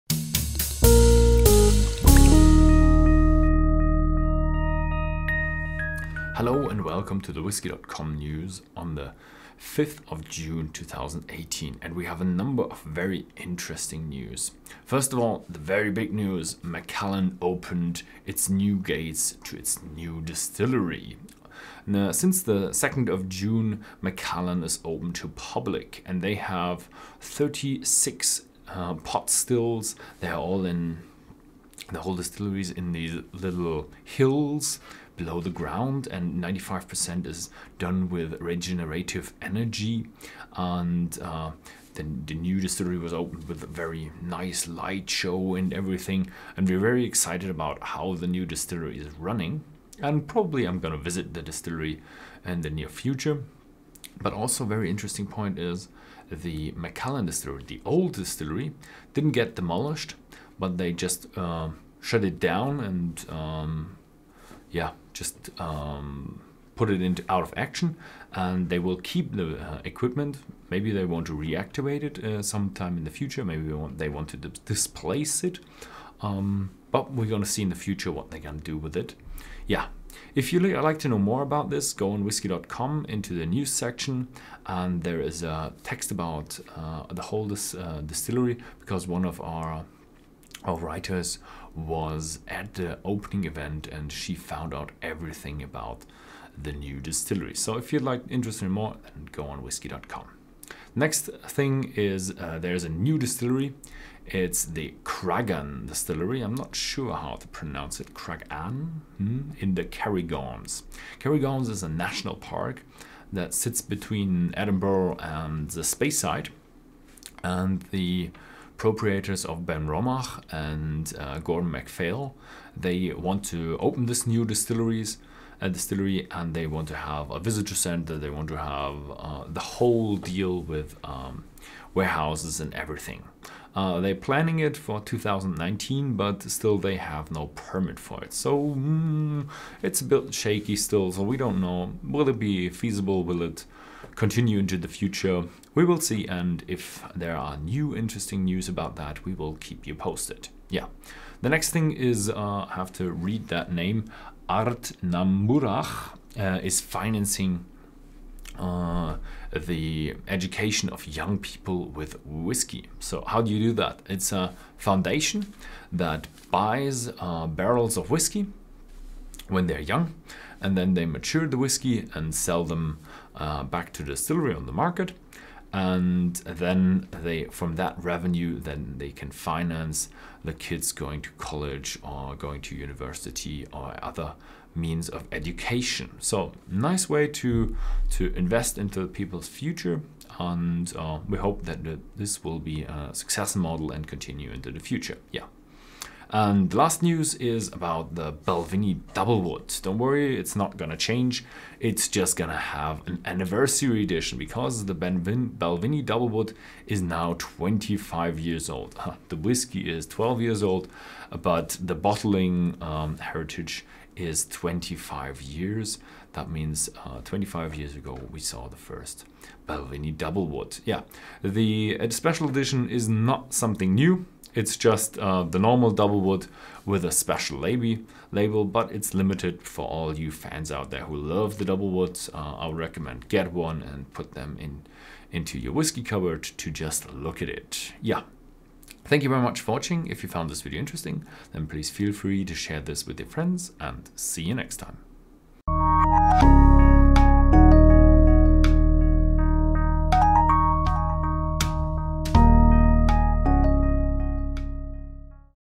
Whisky News June 5th 2018
- New Macallan distillery opened
- Plans of new distillery at Craggan in the Cairngorms
- Ardnamurchan: Education finances with whisky
- Balvenie Doublewood 12 years 25th Anniversary Edition